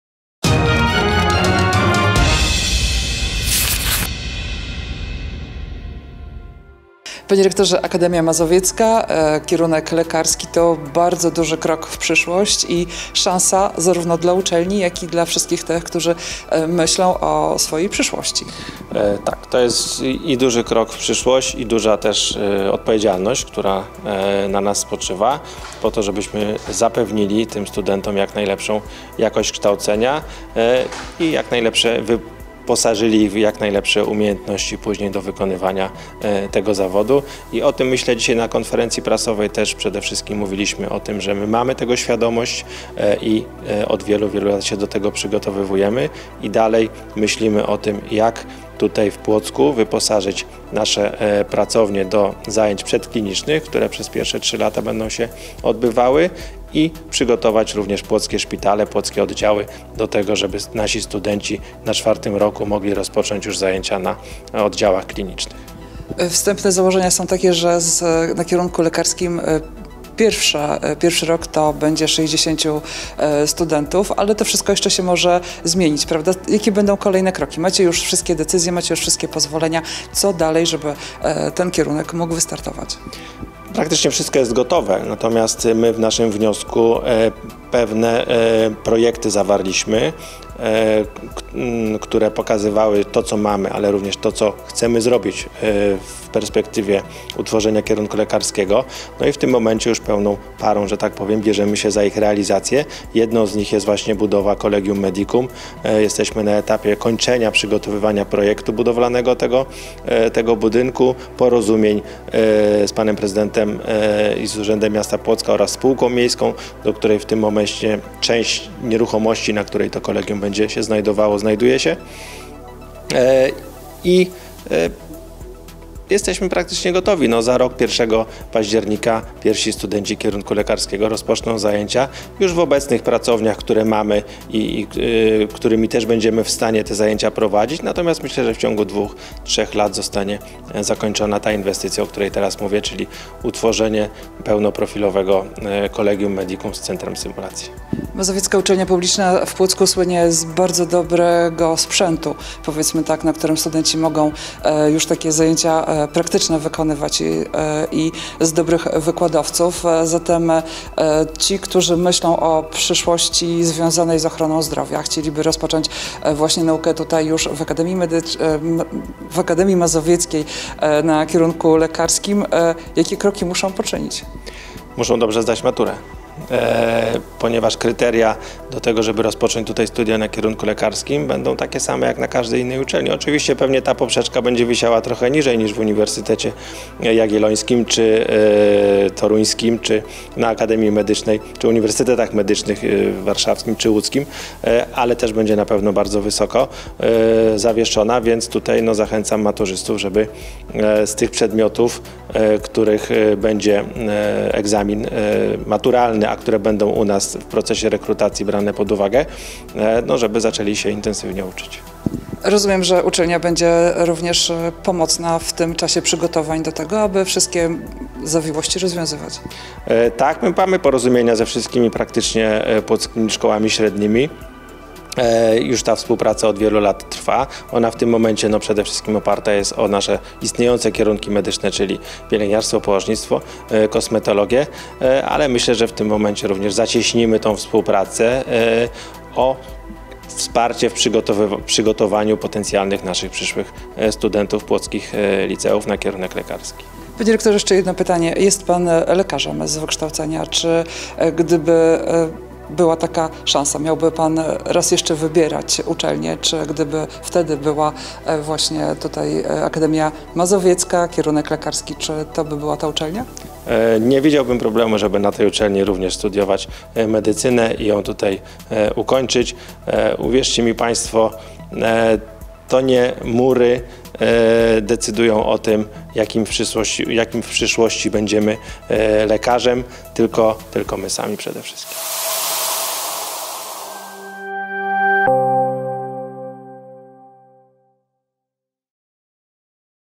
Setka z... Maciej Słodki /rektor Mazowieckiej Uczelni Publicznej w Płocku/
Mazowiecka Uczelnia Publiczna w Płocku to już wkrótce Akademia Mazowiecka w Płocku. Uczelnia otrzymała wszystkie niezbędne pozwolenia na otwarcie kierunku lekarskiego. Więcej na ten temat w rozmowie z Rektorem Uczelni – prof. Maciejem Słodkim.

Chcesz nam coś przysłać lub nas odwiedzić? Zapraszamy:

infoPłockTV
ul. Kolegialna 38i
09-402 Płock
Poniedziałek - Piątek
godz. 9:00 - 17:00